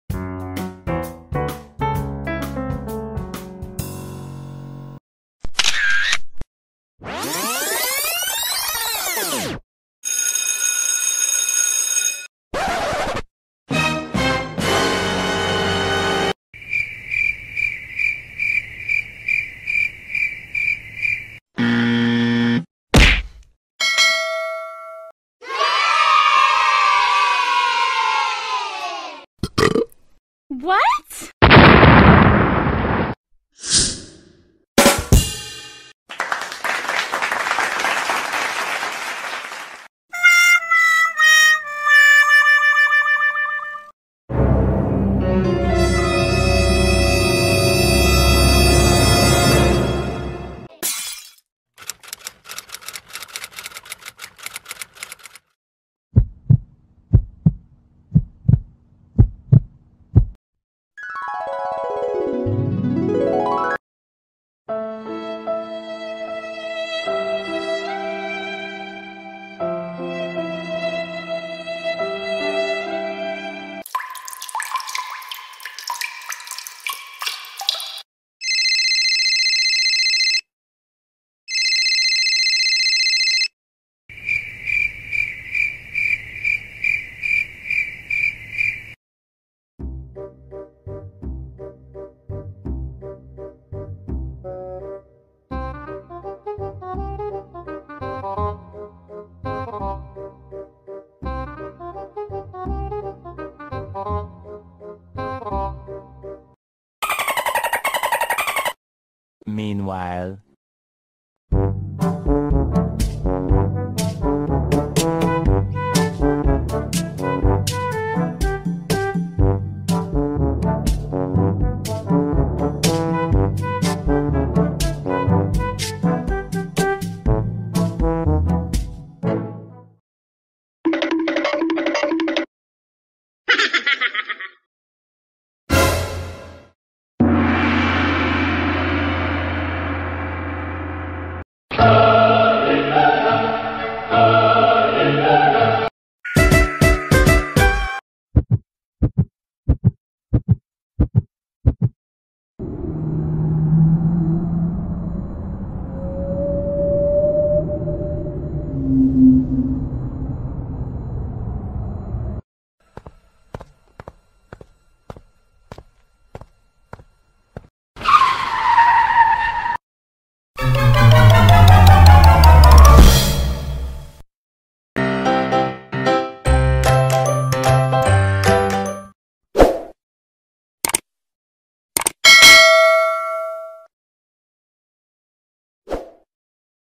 41 POPULAR SOUND EFFECTS FOR VLOGGERS | KAKOSAMO TV
This is the sound effects use by famous youtube vloggers...

shutter sound
Scratch
Cricket sounds
Hugot sound and more...